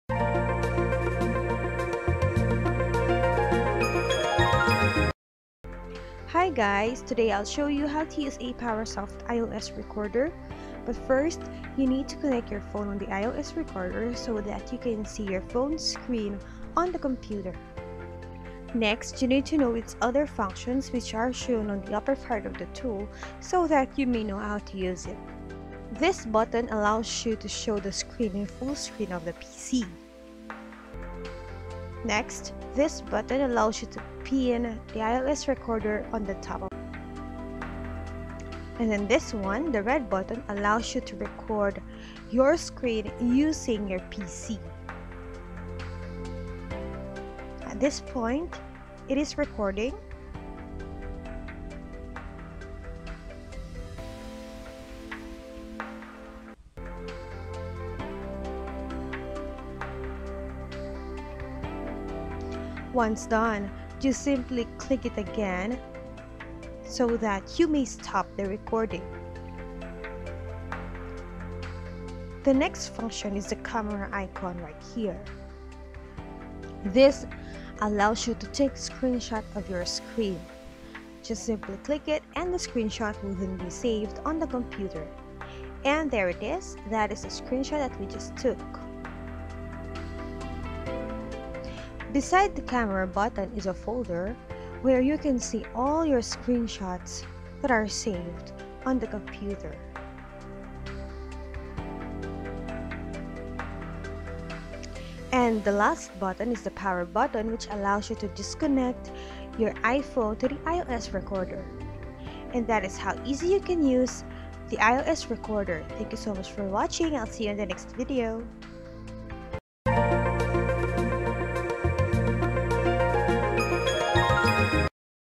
How to use Apowersoft iOS Recorder
This video guide will show you how to use Apowersoft iOS Recorder, you can use it to mirror your iPhone screen to PC, and record whatever displays on the screen.